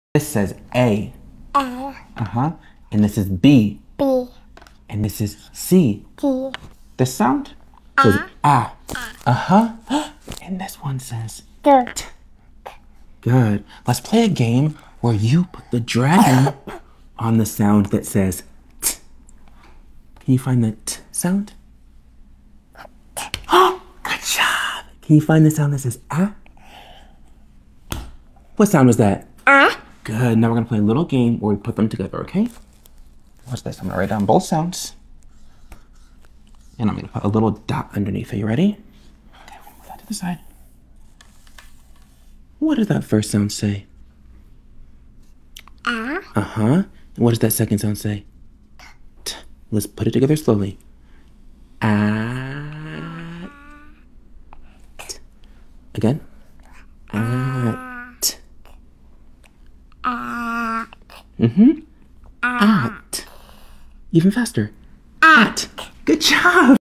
If you're already spending the time with your little one working on literacy skills, get the MOST..
TEACHING ::::::::::| If you're already spending the time with your little one working on literacy skills, get the MOST out of the time by focusing on the letter sounds.